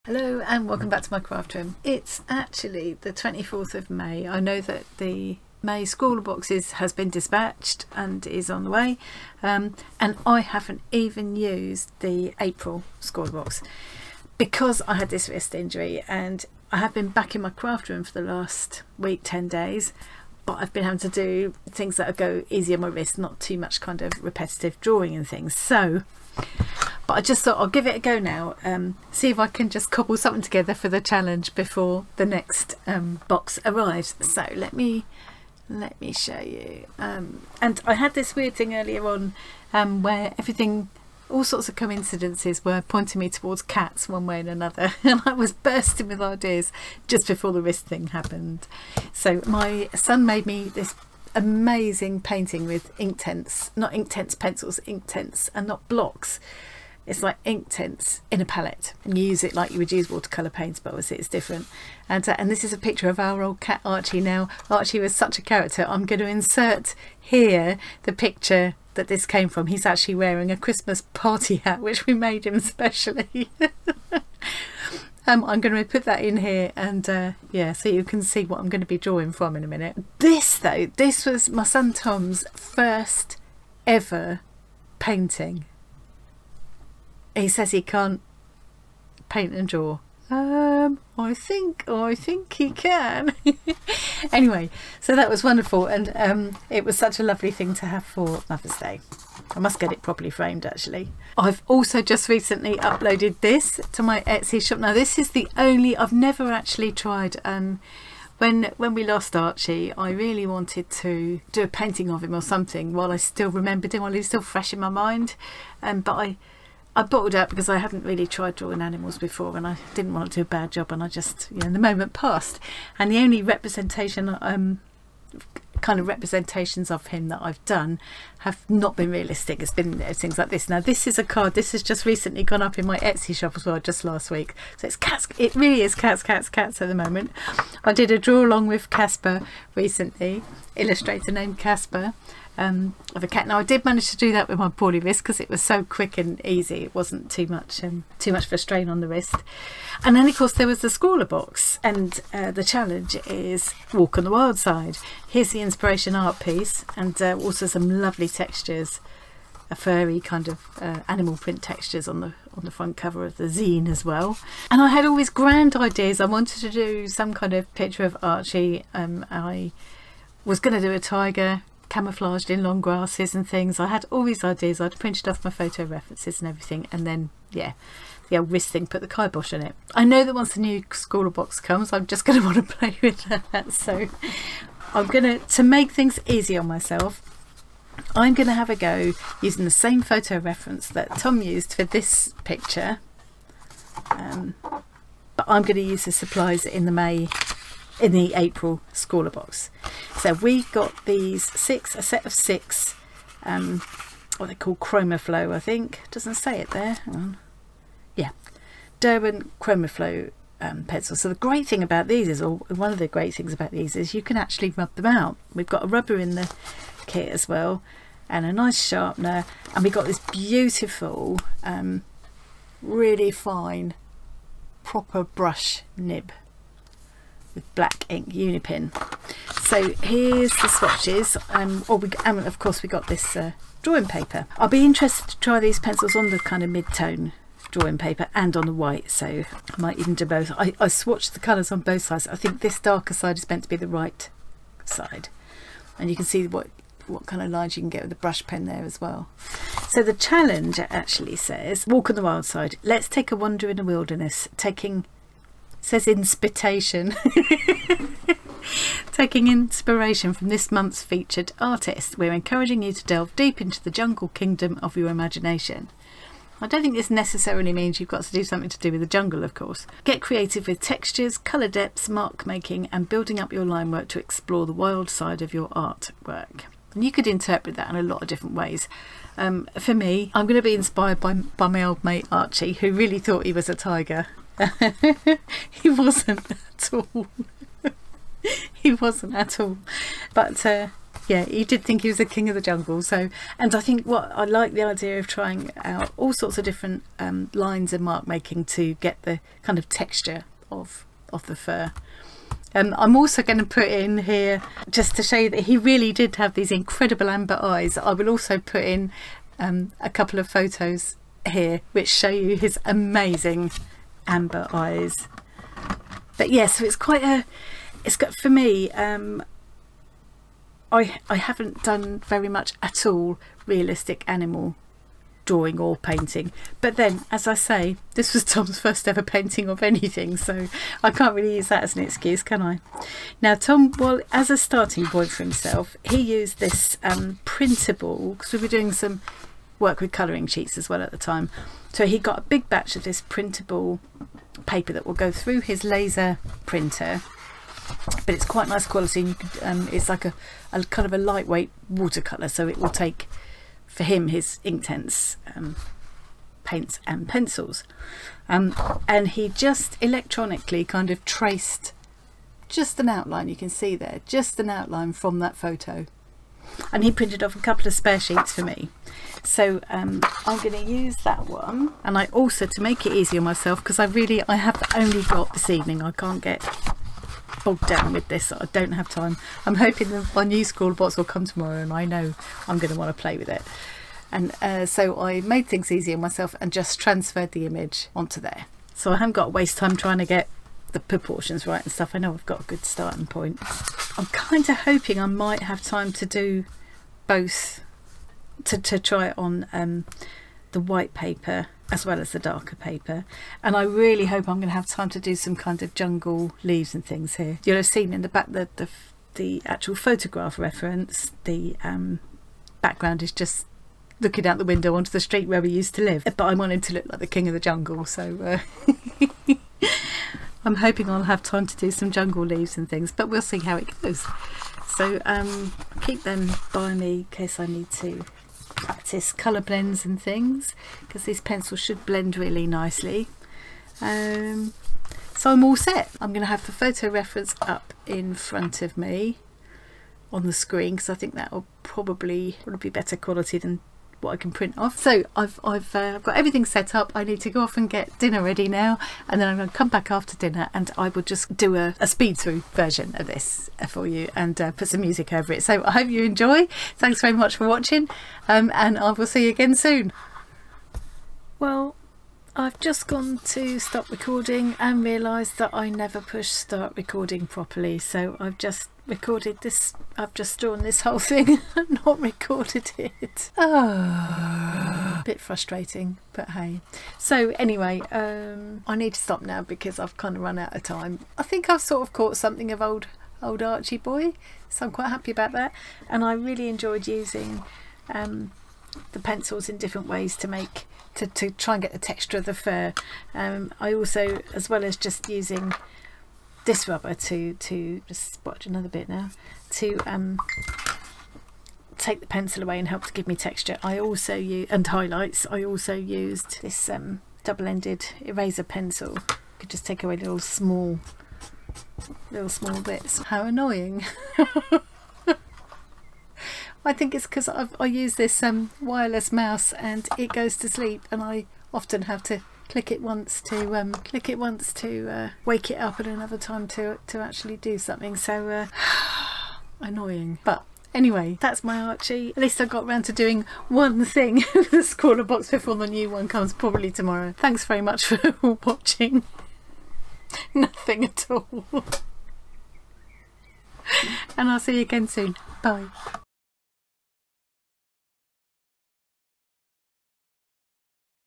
Scrawlr Challenge April 2022
After the wrist injury, this was my first chance to have a go at the April Scrawlr challenge before May box arrives. It went pretty well, until I realised that I'd forgotten to press "Record" so I can't show you the drawing process, but I thought you might like to see how it turned out :)
The supplies in this box included a set of six Chromaflow coloured pencils and the challenge was "A walk on the Wild Side". I really enjoyed blending, layering and erasing these pencils and using them to make different kinds of marks to create the texture of the fur on my old mate Archie.